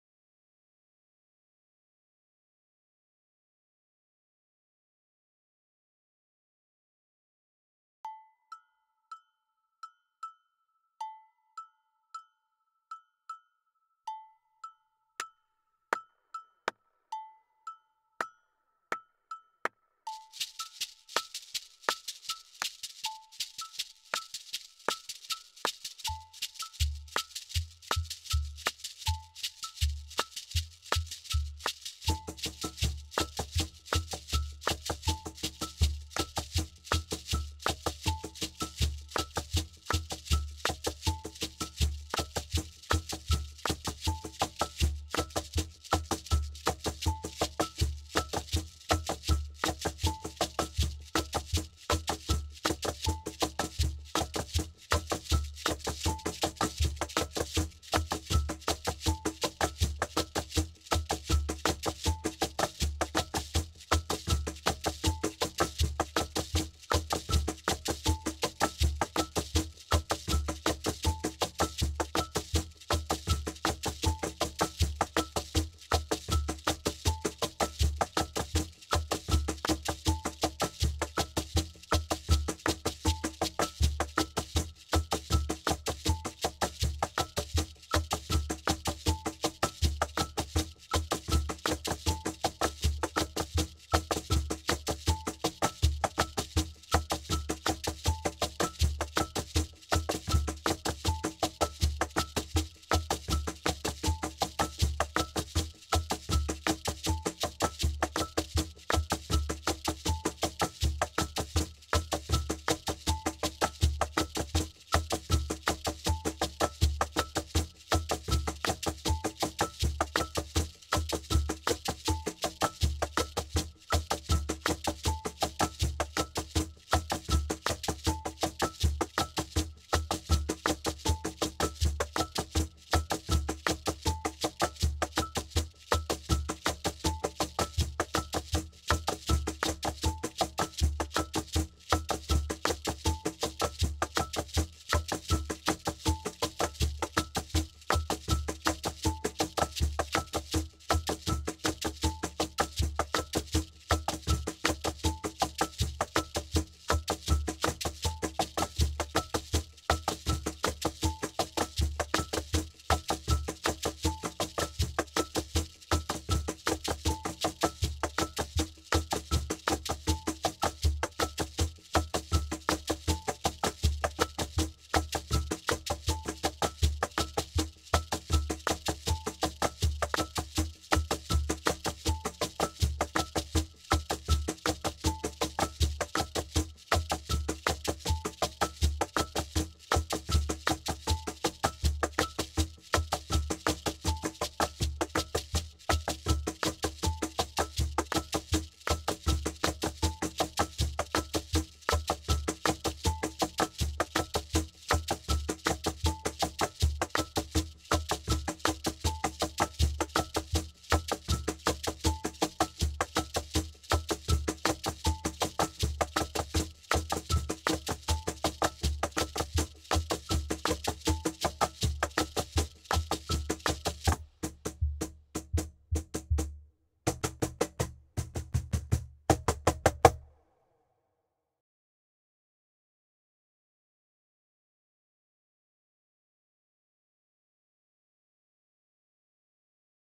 Basic Beats For Cajon - Slow Kpanlogo
This lesson shows you the various patterns of the Kpanlogo Rhythm from Ghana. In a traditional setting these rhythms are played on Kpanlogo drums but they are presented here on cajon.